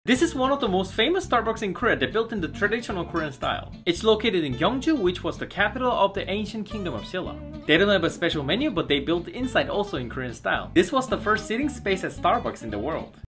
🥇The most famous Starbucks in Korea
Where to travel in Korea except Seoul?

You must visit "Gyeongju"
Gyeongju was the capital of the ancient kingdom of Silla(57 BCE – 935 CE).

The city is so historical that almost every kid in Korea goes there on a field trip. (Including me)

Here's the list to visit.

1. Bulguksa Temple(The most famous place during autumn leaves)
2. Donggung Palace and Wolji pond
3. Seokguram(UNESCO World Heritage)
4. Woljeonggyo Bridge
5. Daereungwon Tomb Complex
6. Cheomseongdae Observatory
7. Gyeongju Gyochon Traditional Village

Except/제외하고는/je-oehagoneun
Capital/수도/sudo
Ancient/고대의/godaeui
Kingdom/왕국/wang-gug
Historical/역사적/yeogsajeog
Field trip/현장 학습/hyeonjang hagseub
Autumn leaves/단풍/danpung
Heritage/유산/yusan

😎Follow for more Daily Dose of Korean!
💾Please Save and share for later